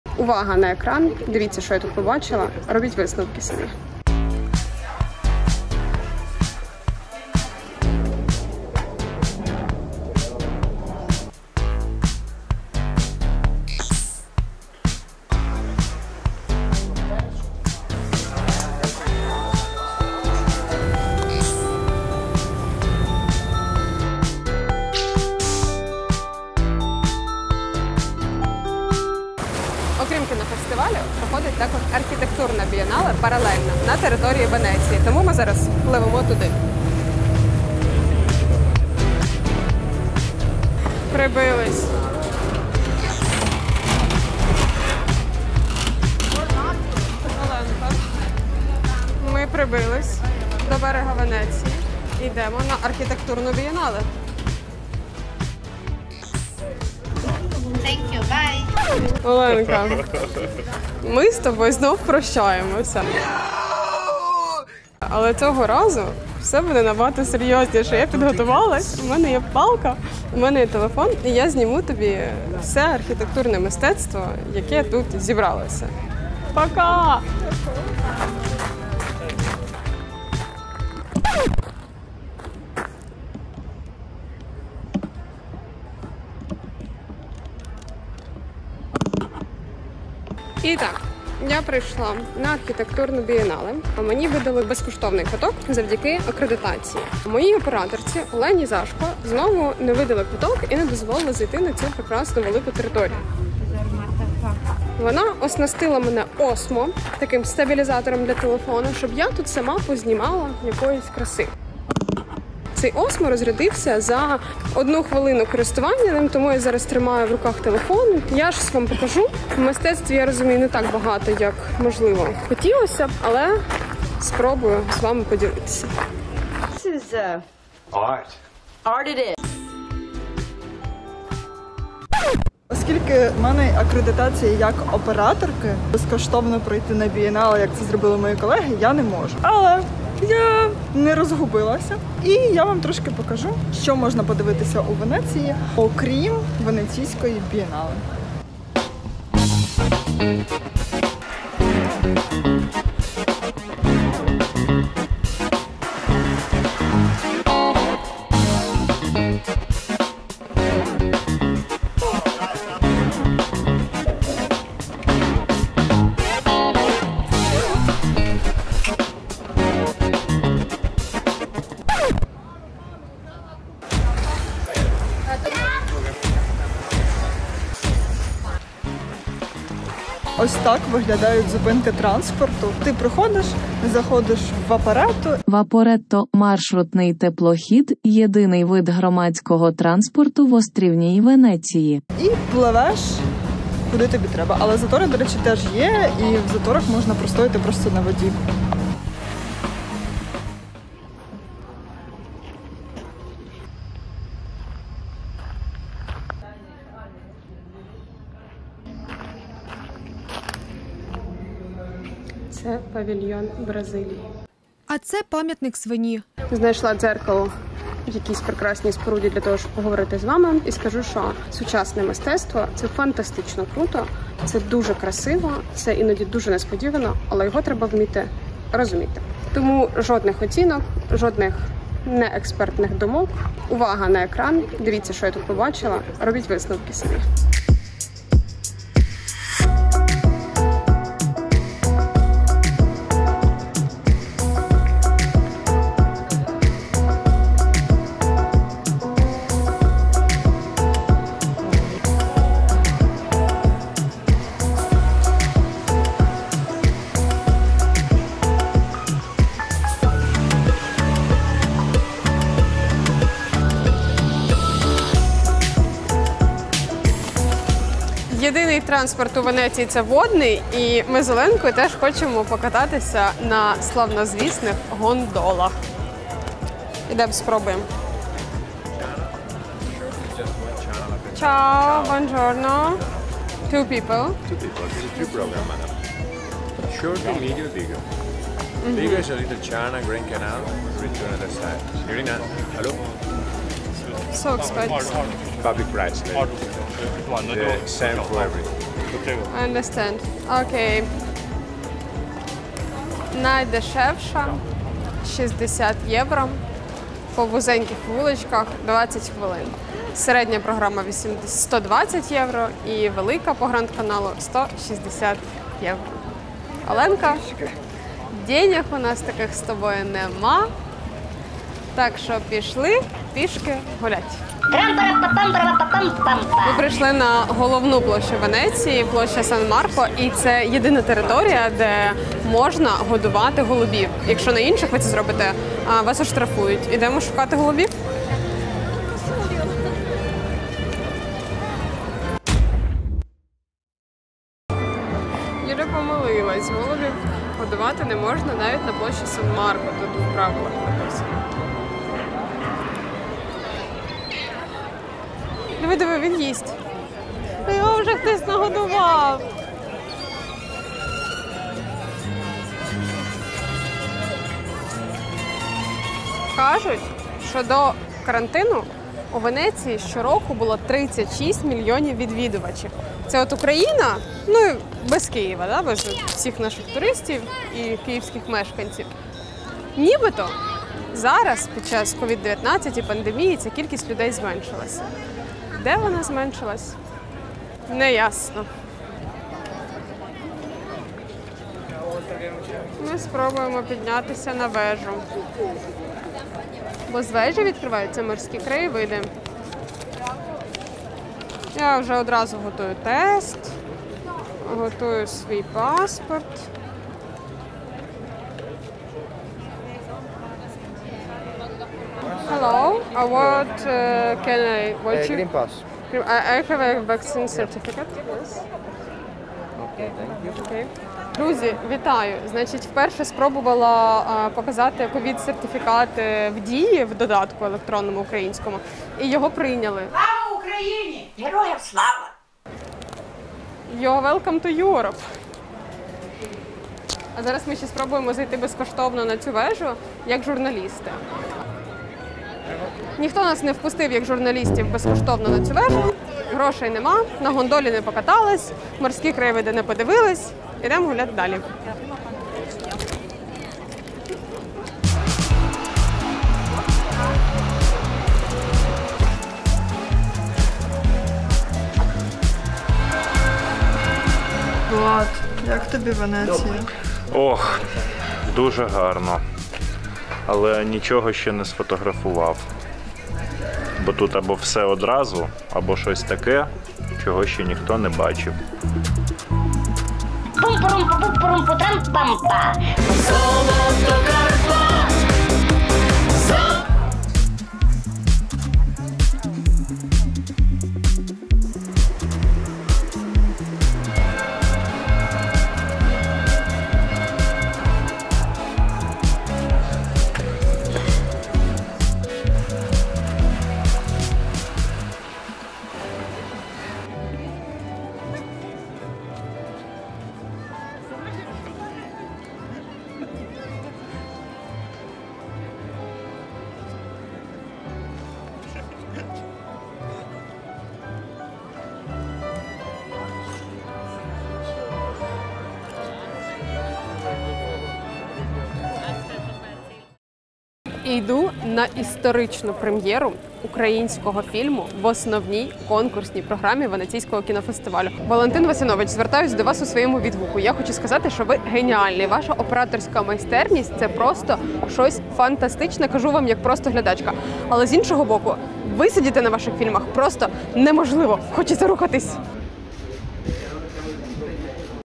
Гондоли, канали, архітектурна бієнале | тревел-випуск | Кіноспеції з Венеції
У третьому випуску влогу з Венеції команда Суспільне Культура відвідала Венеційську архітектурну бієнале. Проте потрапити туди змогли не всі. Юля, Олена та Влад підкорюють місто на воді — спробують покататися на гондолах, покажуть площу Святого Марка та розкажуть про Венецію у період COVID-19. Багато краси та мало кіно (знову не потрапили на сеанс) — третя серія «Кіноспеції з Венеції».